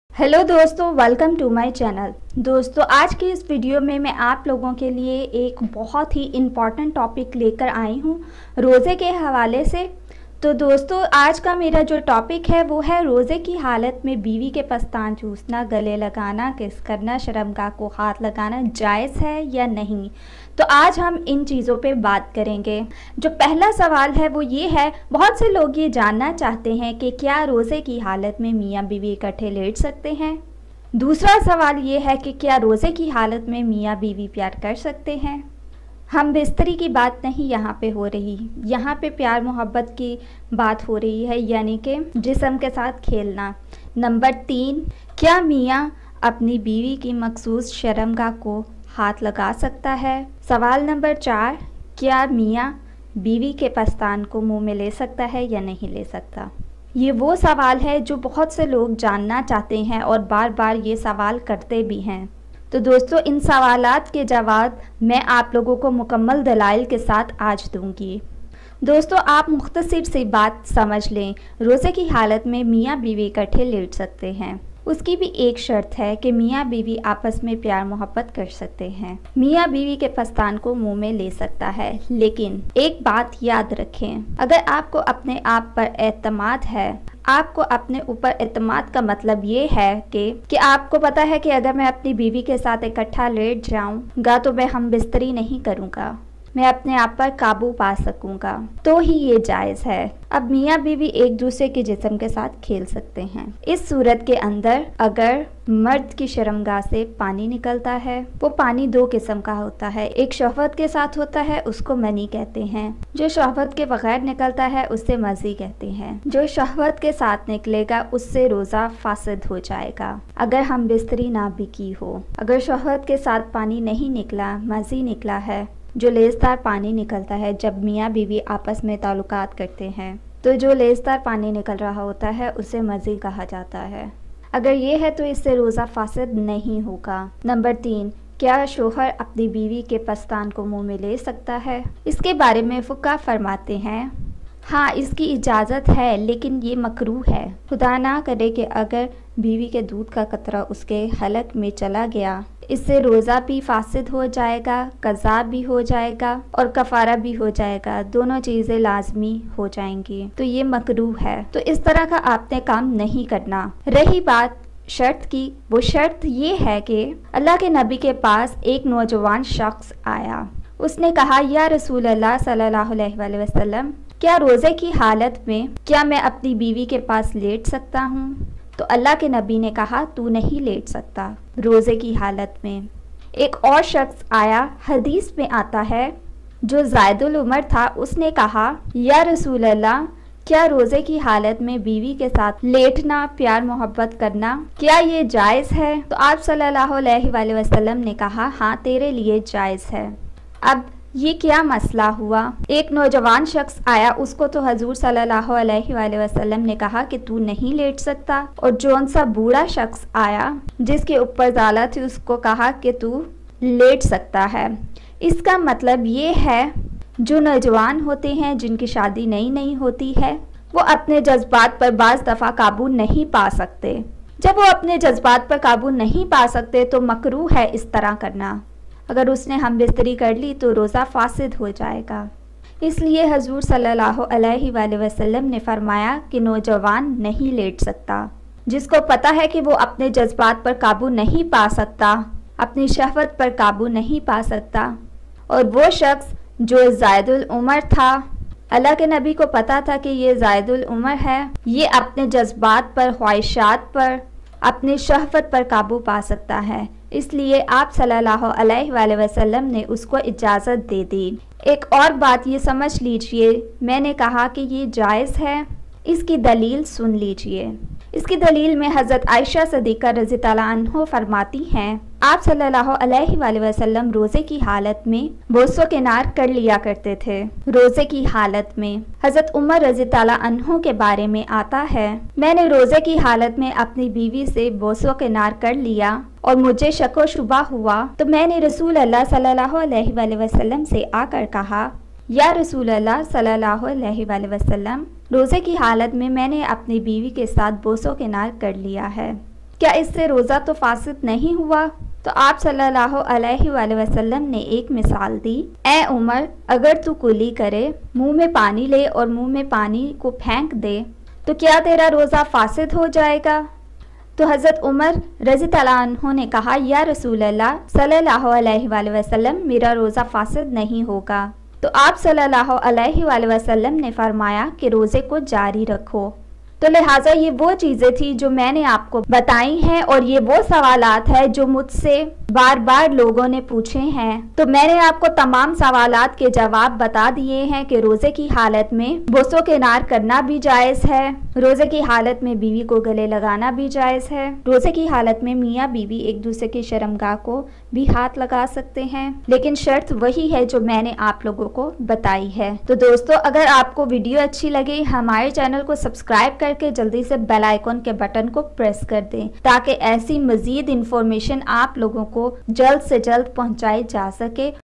Rozay Ki Halat Men Bv Ko Chomna | Roze Men Biwi Ke Sath Sona or Bosa Lena Jaiz hai in Urdu and Hindi
Dosto aj ki is video men jo hmra topic ha woh yeh hai k Roza kin btao sy makro hta hai. Kya roze men biwi k sath sona or biwi ka bosa lena jaiz hai. Kya rozay ki halat men bv ki sharamgah ko hath lgna jaiz hai k nai. Kya shohar biwi k pistan ko roze ki hath men mou men ly skta hai k nai. So aj hm in sari btao ko janne k lye is pay bt kryn gy or quran or Hadis men kya kaha gya hai is bary men woh b janty hain. Dosto hmri is video ko end tk zaror dkhyn or jo b ap k zehan men is sy related sawal ho. Ap log poch skty hai, Rozay ki halath men shokar biwi k pistan ko choos ska hai likan us k lye aik shart rkhi gai hai Or rozay ki halat men shohar bv ki sharamgah ko b hath lga skta hai. Bv ko galley b lga skta hai us ka bosa b ly skta hai. So is k lye kch shartain rkhi gai hai. Un pay aamal kr k ap log kr skty hain.